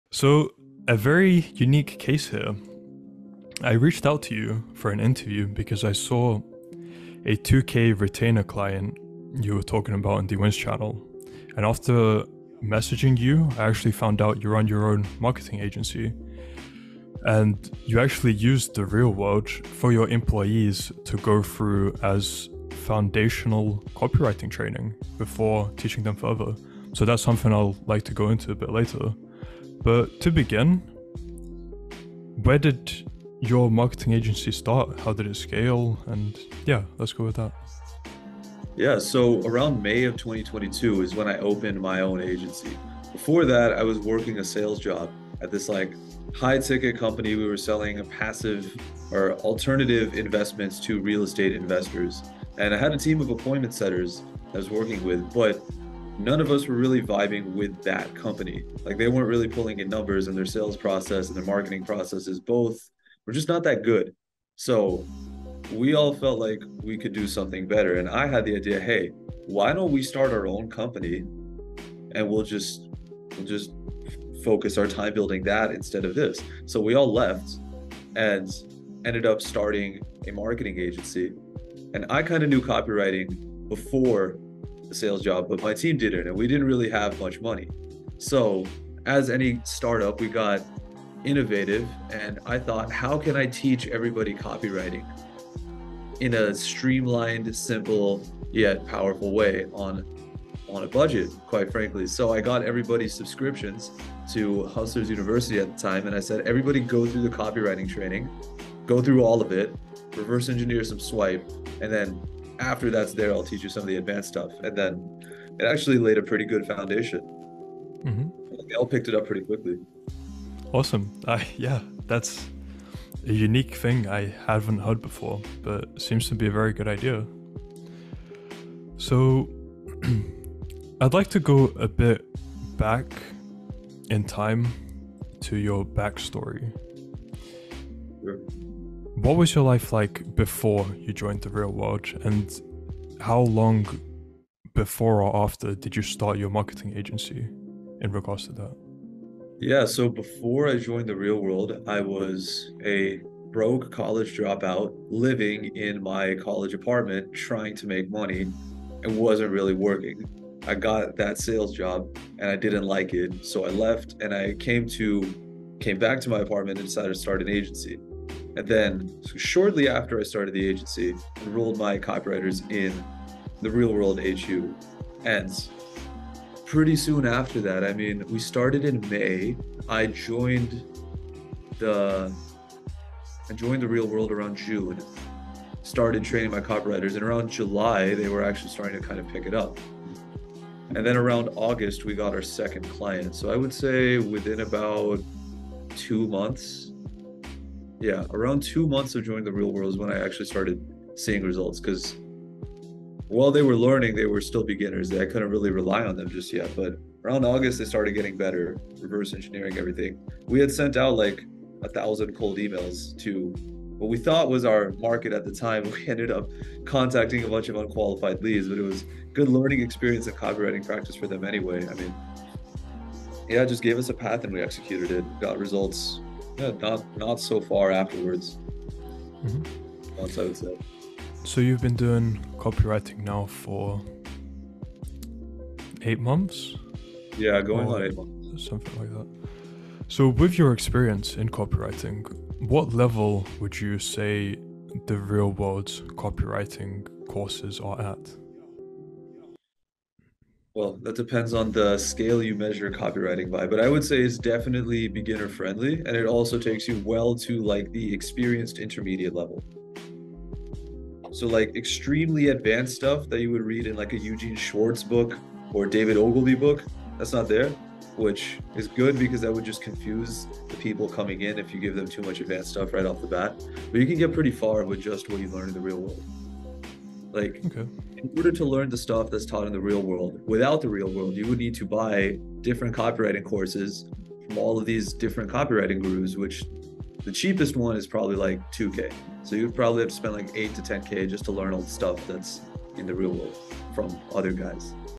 The Real World by Andrew Tate review - Never seen before success
Shahraja shares hsi unique perspective on The Real World and how he used the program to level up his copyrighting agency.